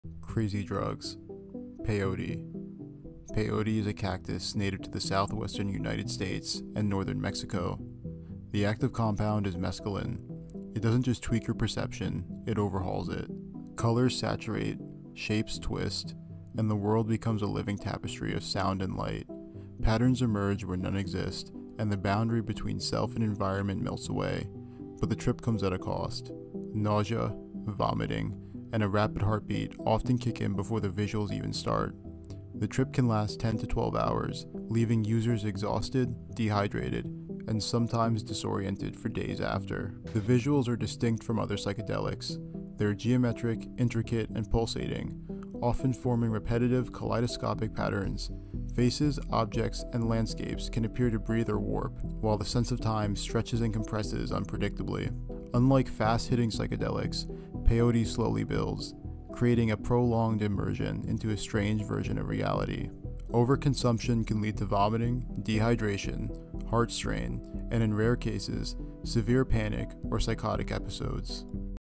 Crazy and Obscure Substances: Peyote aka Lophophora williamsii
Today we explore a substance known Peyote aka Lophophora williamsii